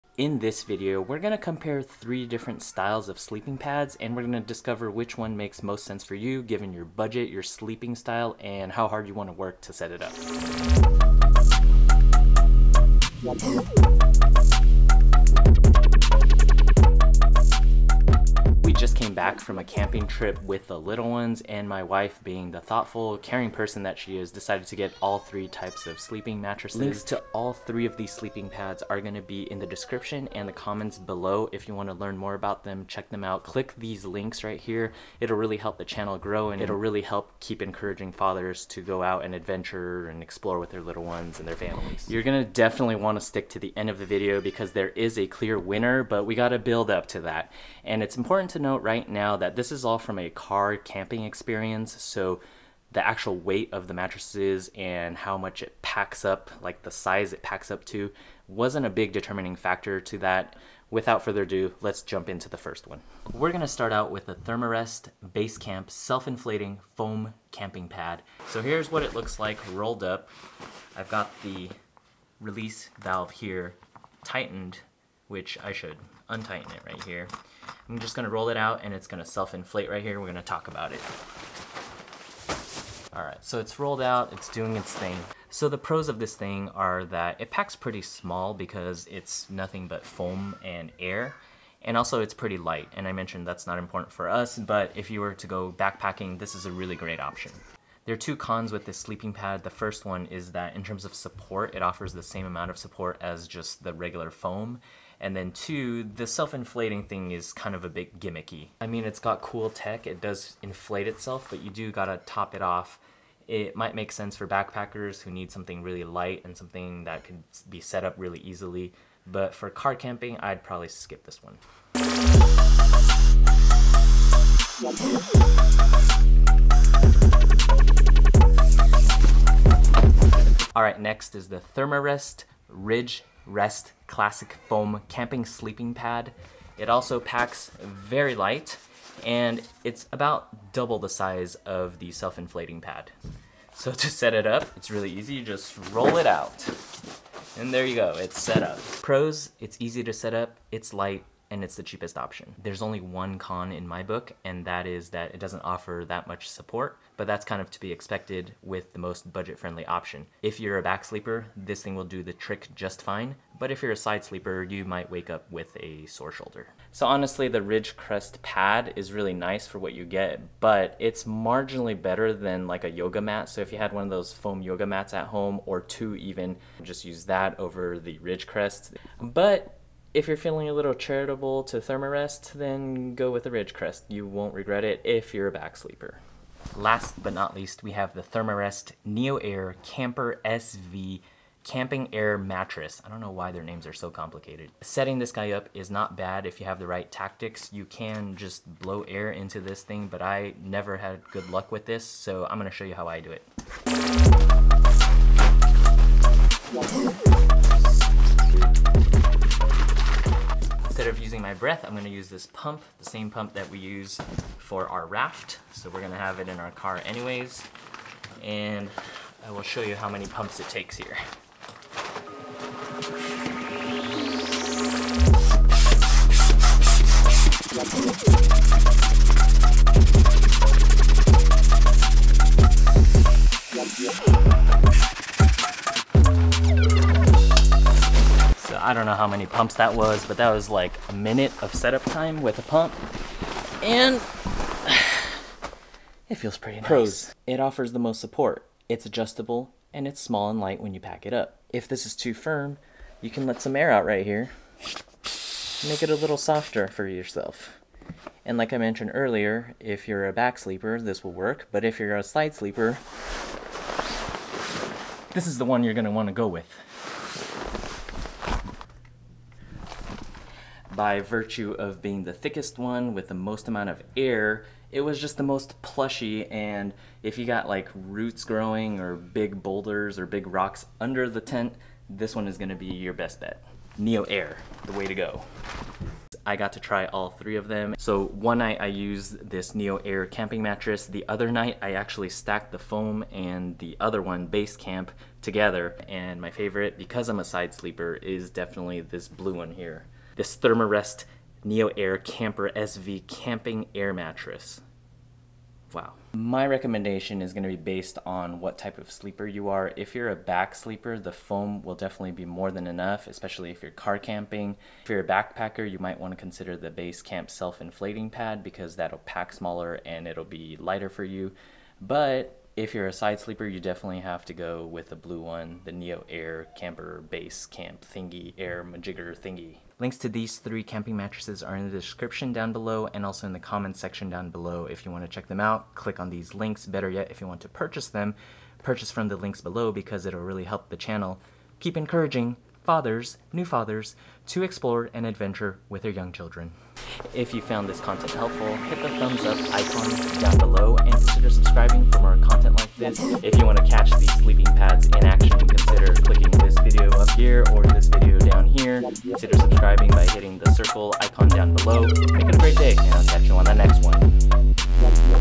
BEST THERMAREST FOR CAR CAMPING - basecamp self-inflating VS RidgeRest Classic VS NeoAir Camper SV
Side sleepers - I've done the testing so you don't have to on the Therm-a-rest lineup. No marketing BS and gets to the important stuff fast. Get a good night's rest to maximize adventuring with your little ones!

Basecamp Self-Inflating Foam Camping Pad
RidgeRest Classic Foam Camping Sleeping Pad
NeoAir Camper SV Camping Air Mattress

Basecamp Self-Inflating Foam Camping Pad: If you’re still car camping or base camping in the frontcountry with your backpacking pad, introduce yourself to the luxurious and roomy BaseCamp sleeping pad. This warm and comfy pad features 2 inches (5 cm) of premium self-inflating foam, offering campers a 6.0 R-value for better nights under the stars. Thoughtful face fabrics are light and soft for your comfort. The versatile self-inflating pad features the high-performance WingLock valve, which has three times the airflow rate of the classic valve thanks to a larger opening and one-way inflation. Like all Therm-a-Rest pads, the BaseCamp is made in market to ensure quality and minimize environmental impact. The pad includes a go-anywhere stuff sack.

Classic Foam: Virtually indestructible, the RidgeRest® Classic mattress is a revival of the original. Using our most durable foam this mattress is sure to last many a year, delivering exceptional value to campers looking for basic comfort.

NeoAir Camper SV: The Therm-A-Rest NeoAir Camper SV Camping Sleeping Pad for spring, summer, and fall camping brings the comforts of home to the campground. Extra thick and comfortable, this mattress adds a padded layer of warmth between sleeping bag and ground for a cozy’s night sleep. Proprietary reflective ThermaCapture technology traps body heat within the pad, while the WaveCore baffled constructin minimizes heat loss. With an R-value of 2.2 it’s ideal for three-season camping (R-value measures the material’s ability to insulate; the higher the R-value, the warmer the sleeping pad). Mattress inflates easily, using a SpeedValve that pulls in surrounding air to amplify the breath, quickly inflating the mattress for quick setup in a tent; auto-reversing the valve deflates the mattress quickly. The Therm-A-Rest NeoAir Camper SV Camping Mattress is perfect for creating comfort in outdoor environments. Available in three sizes: Regular measures 72x20x3 inches inflated, packs to 10x5.5x5.5 inches, and weighs 29 ounces; Large measures 77x25x3 inflated, packs to 12x6x6 inches, and weighs 37 ounces; X-Large measures 77x30x3 inflated, packs to 15x6x6 inches, and weighs 45 ounces. Stuff sack and repair kit included. Manufacturer’s limited lifetime warranty. Made in the USA.

0:00 intro
1:00 Basecamp Self-Inflating Foam Camping Pad
2:08 RidgeRest Classic Foam Camping Sleeping Pad
3:06 NeoAir Camper SV Camping Air Mattress
4:46 Verdict, Recommendation, Final Thoughts